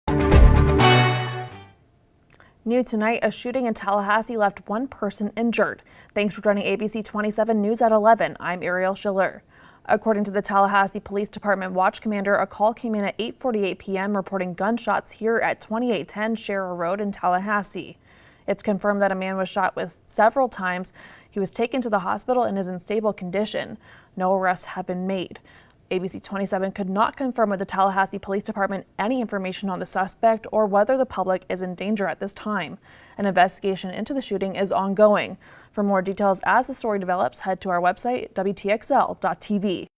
Tallahassee Police Department investigating shooting incident on Sharer Road
The Tallahassee Police Department confirmed Sunday that a shooting occurred at 2810 Sharer Road.